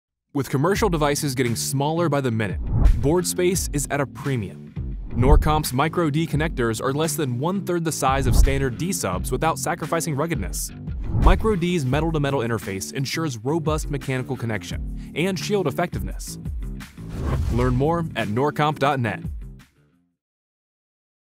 MICRO-D Miniature D-Subs connectors offering commercial MILSPEC equivalents.
MACHINED MICRO-MINIATURE CONNECTOR SYSTEM
The compact design of the NorComp Machined MICRO-D Connector offers 1/3 the size of a standard d-sub. Offering metal to a metal interface for robust mechanical connections, this high reliability, space-saving design is ideal for applications such as: industrial Robotics, test & measurement, hand-held & portable LCD/LED, and laboratory meteorology.

FEATURES
Machined Contacts
COTS
Available In 3 Industry Standard Sizes | 9, 15, 25
3 Termination Variations Available: Cable Mount, Vertical Solder, & Right-Angle Solder
1/3 The Size of a Standard D-Sub
Metal to Metal Interface for Robust Mechanical Connections & Shield Effectiveness
2-Amp Contact Rating / 60V AC Voltage Rating / -40°C To +125°C
SERIES
Solder Cup
Vertical Solder
Right Angle Solder